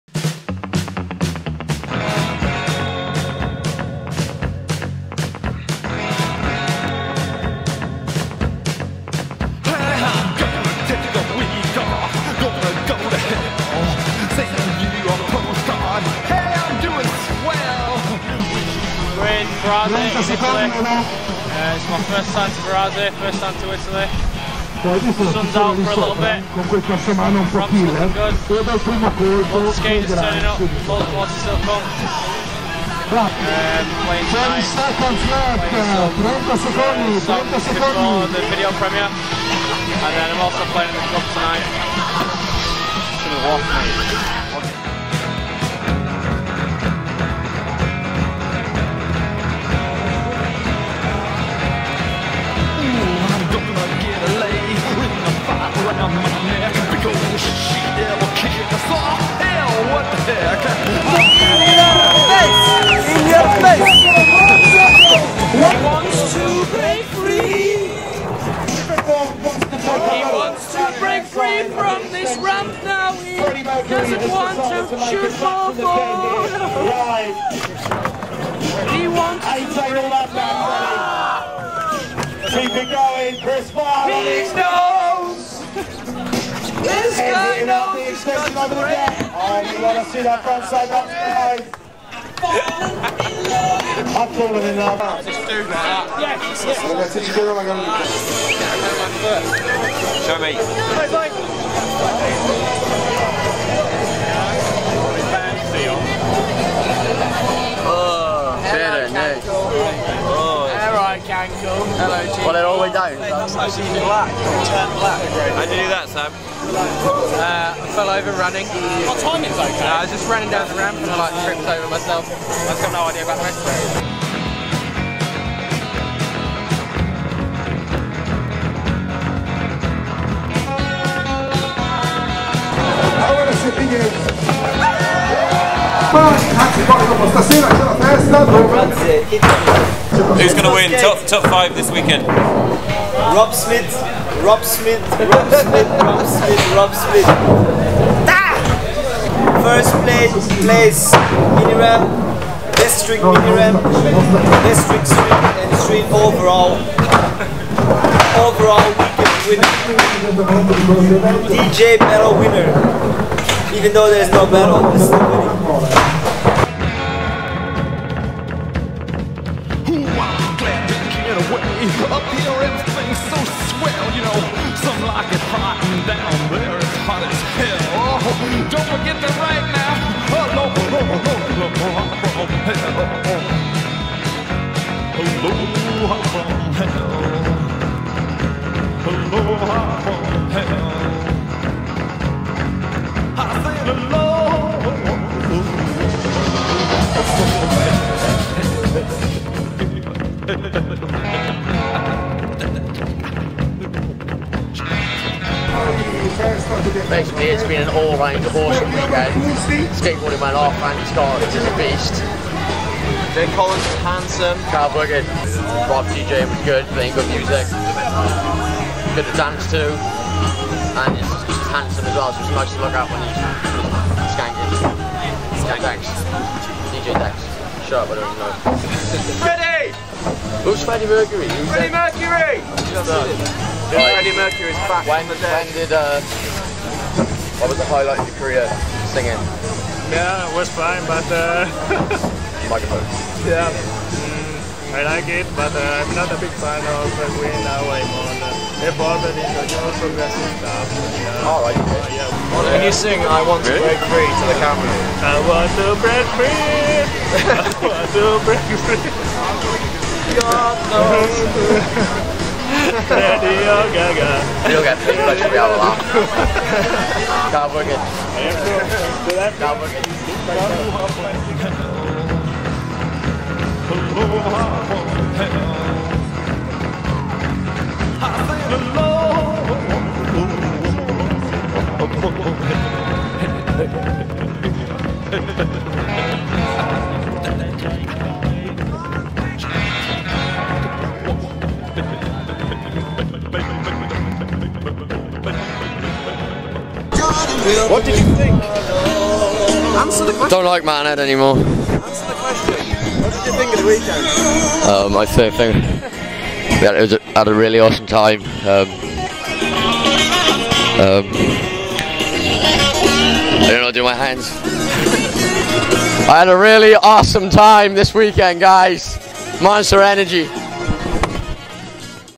Socks' New Underwear....Vans Spring Classic with Monster Energy
A fine weekend spent with the Monster Energy Skateboard team in Varazze Italy for the Vans Spring Classic, the weather held out, the comp went off, DJ Dogger (AKA Rob Smith) was up in the club and Freddy Mercury turned up and Socks went home with some new underwear.